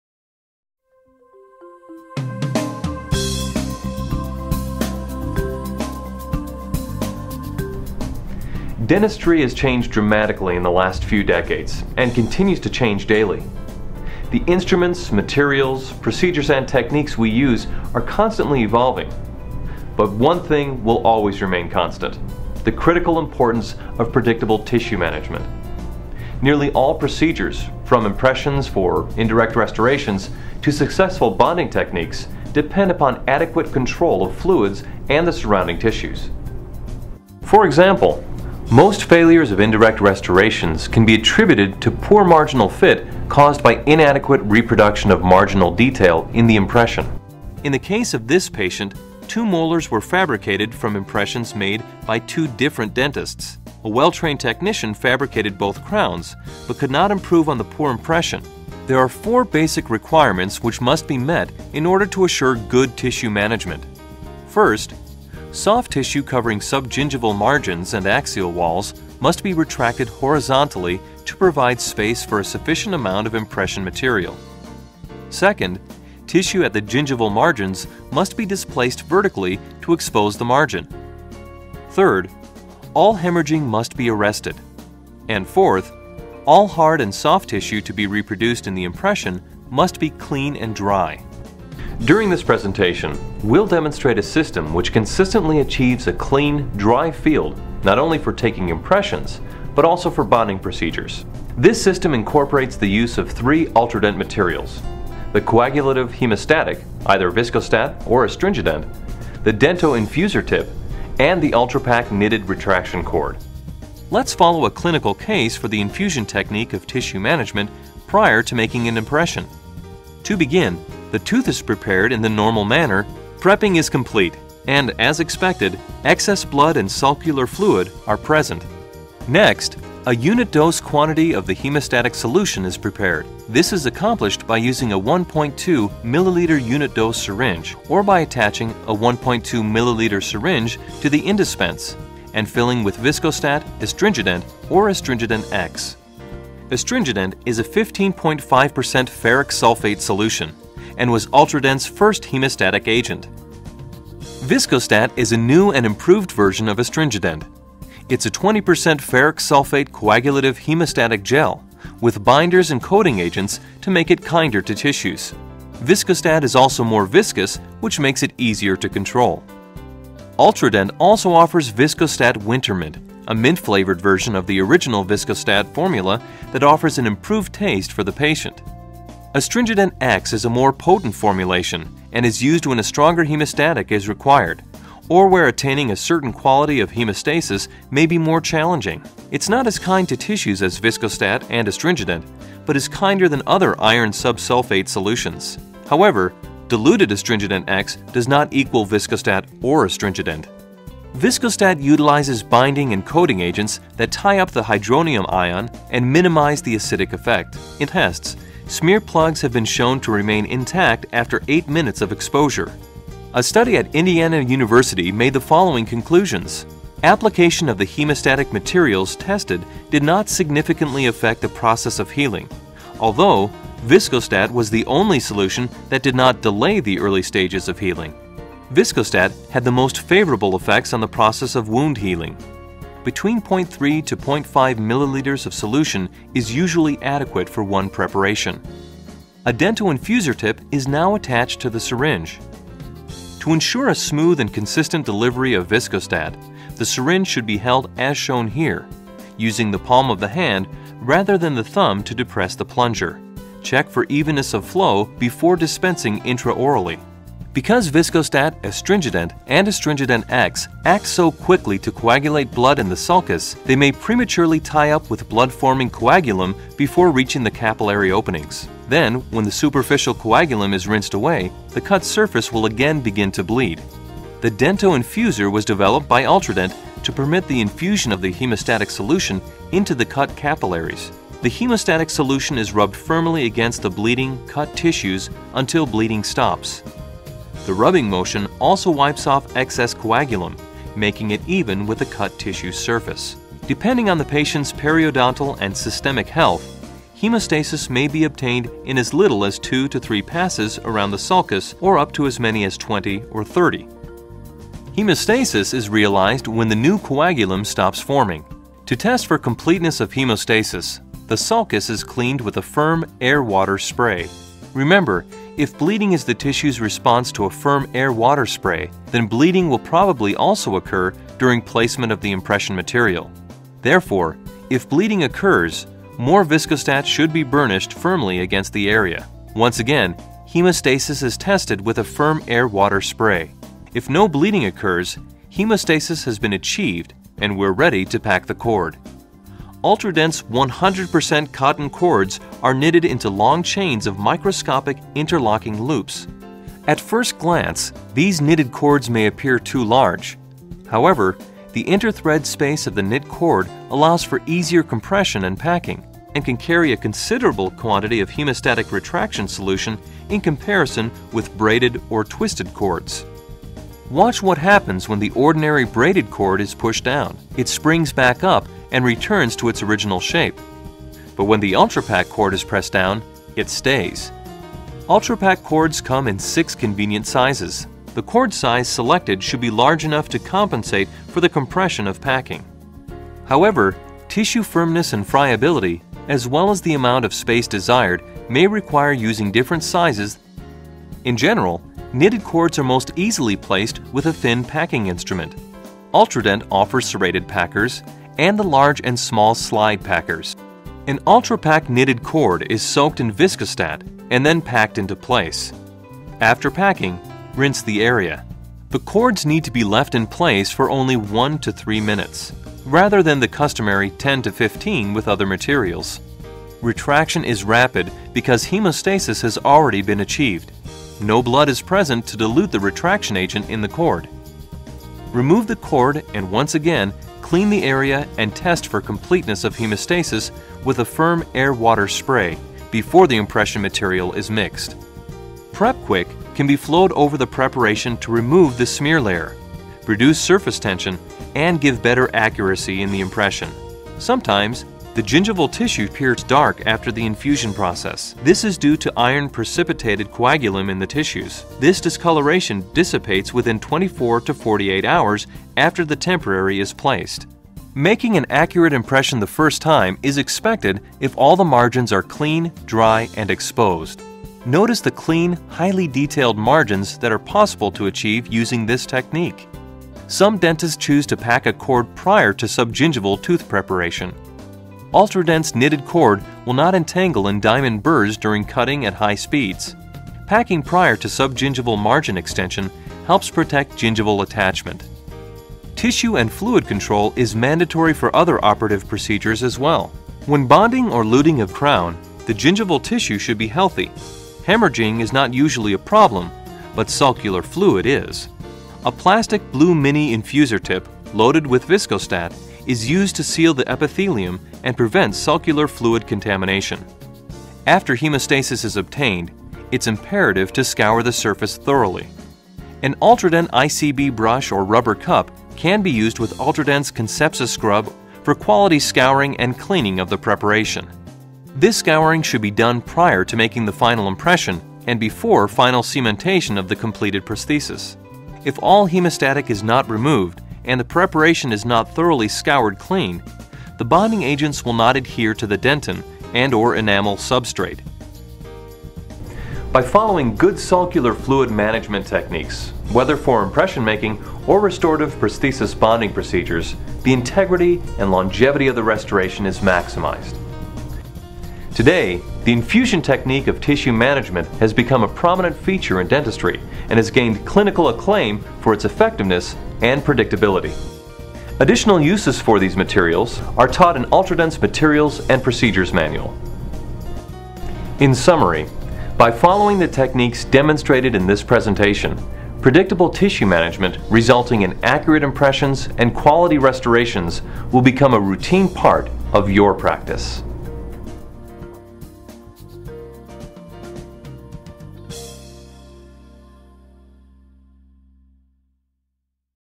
Ultradent Products, Inc.'s Tissue Management Technique
Tissue Management is the key to quality direct and indirect restorations. Unparalleled tissue management starts with rapid, profound hemostasis. To control bleeding and sulcular fluid, no one offers a more complete line of solutions.